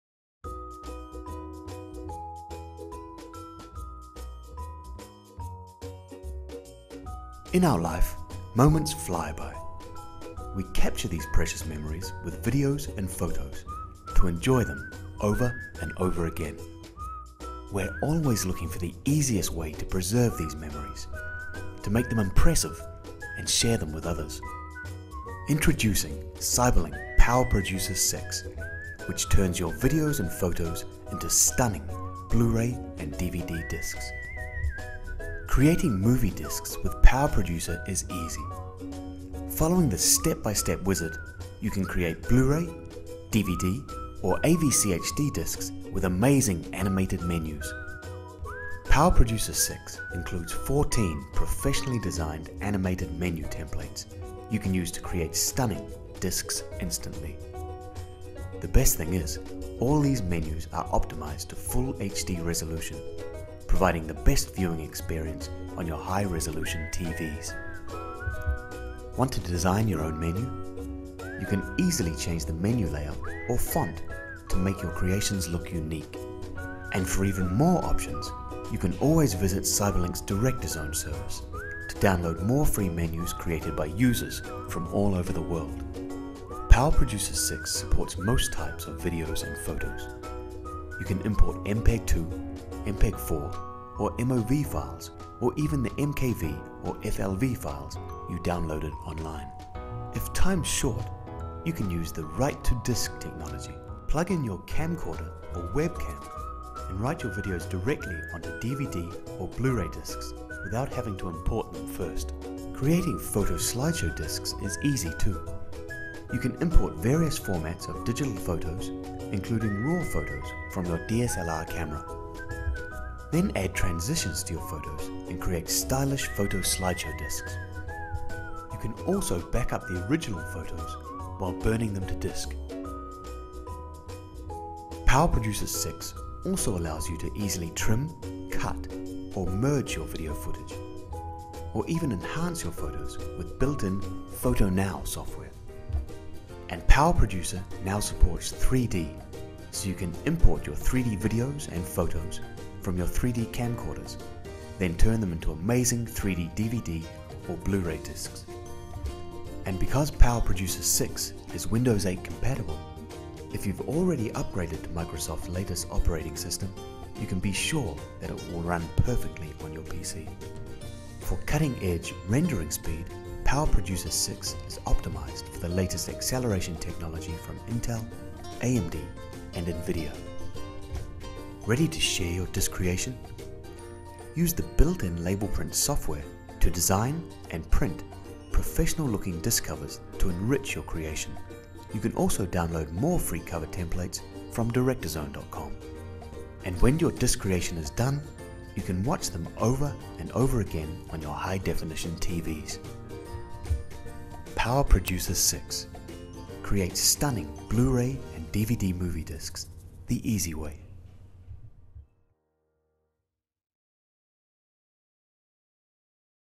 CyberLink PowerProducer 6 Intro Vidéo (French)
PowerProducer contient tout ce dont vous avez besoin pour transformer vos photos et vidéos en disques Blu-ray et DVD hollywoodiens. Il s'accompagne d'outils de gravure complets, prend en charge les derniers formats et produit les vidéos plus rapidement que jamais. C'est le logiciel de création de disques essentiel pour votre PC Windows 8.

PowerProducer contient également des modèles de menus professionnels et d'un logiciel de création et d'impression de pochettes.